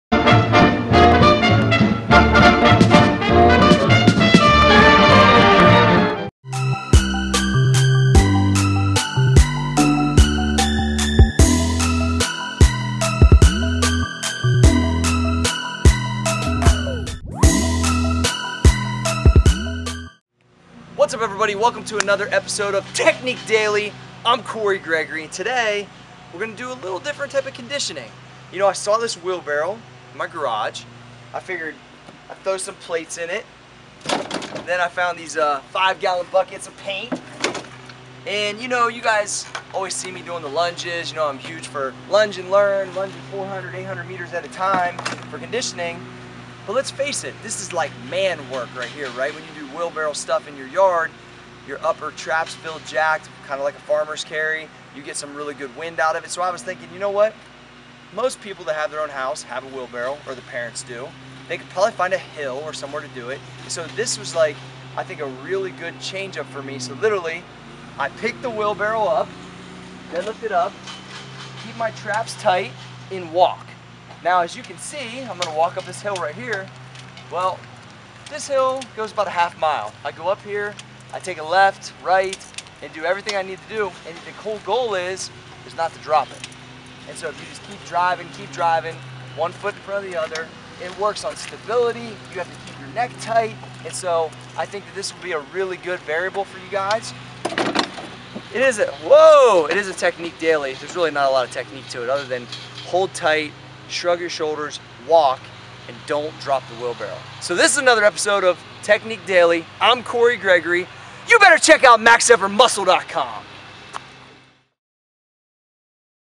Farmers Carry Wheelbarrow Variation | Technique Daily | Cory Gregory
On this episode of Technique Daily, Cory Gregory switches up the conditioning and does a wheelbarrow variation of a farmer's carry! Load up a wheelbarrow with some weights and some gallons of paint and get to it!